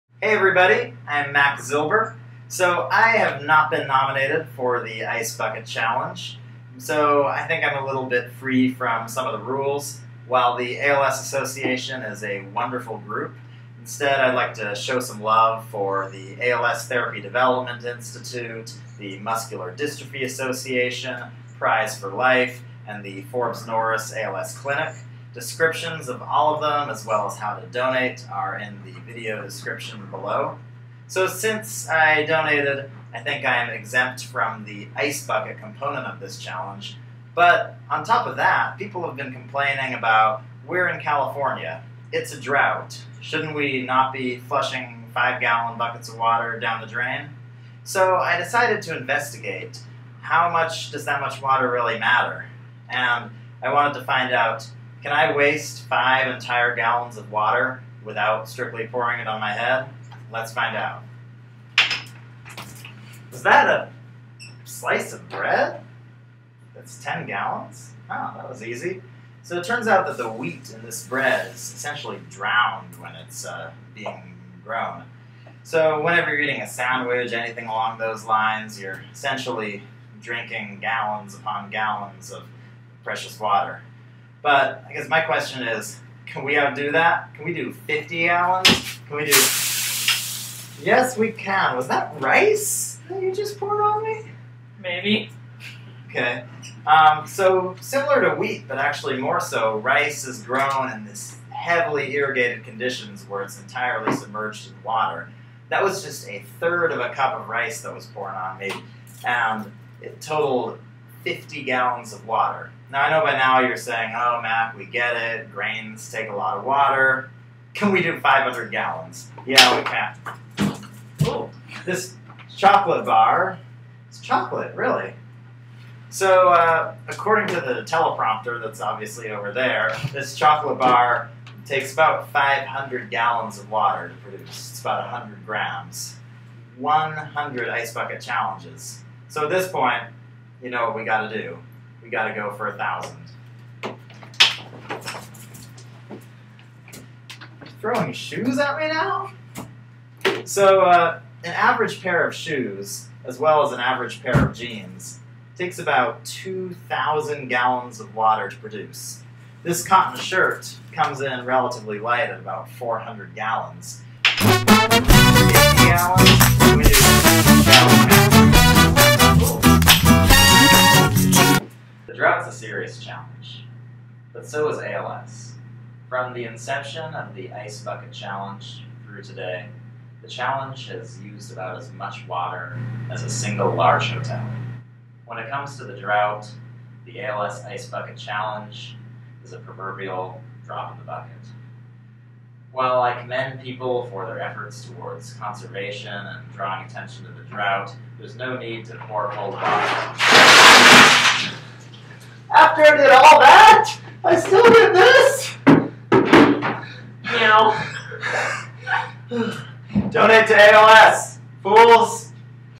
Mac pours 5,555 gallons of "water" on his head
This video is dedicated to my mother, Carla Zilbersmith, who passed away from ALS in 2010. You can read her thoughts on ALS - far more eloquent than mine - at her blog carlamuses.blogspot.com. Scroll down for information about the ALS organizations mentioned in this video.

Note - because this video is made for humorous purposes, there's not enough time in the video itself to fully flesh out the argument for why the Ice Bucket Challenge doesn't have a meaningful effect on the drought. These numbers are all very rough and back-of-the-envelope, but California is estimated to have lost over 180,000,000 acre feet of water (that's acres of water one foot deep) from this drought. We withdrew 42,000,000 acrefeet of water from the ground in California in 2010. The entire icebucket challenge in California has accounted for somewhere in the neighborhood of three acre feet. Not three million, not three thousand. Just three acre feet of water.

I say this not to be glib, but because it really matters if we're going to understand the drought to understand that our water footprint has to do with how we eat, how we shop, and how we consume electricity more than it has to do with how long we wash our hands. Personal conservation is important, but California uses about as much water growing just alfalfa, almonds, and pistachios each year as it uses on the entire public water supply each year (7.5 million acrefeet in both cases). By some estimates, as much as 80% of California's developed water goes toward agricultural purposes. The public water supply is tied with electricity generation for a very distant second place.

ABOUT THE ORGANIZATIONS MENTIONED IN THE VIDEO:

The ALS-TDI is an organization primarily dedicated to scientific research to end ALS.

Prize4Life is an innovative non-profit that designates large prize-based rewards to tangible steps forward in understanding and curing ALS. to learn more.

The MDA is dedicated to helping not just ALS, but all Muscular Dystrophy ailments. The MDA has a real impact of the quality of life of current people suffering from ALS, providing materials and logistical support.

Finally, very near and dear to my heart, the Forbes Norris ALS clinic in San Francisco is one of the best ALS treatment and research clinics in the country. They're remarkably nice, smart, and funny people, and if you're going to give to ALS research, this is not a bad way to cut out the middleman and give right to the source.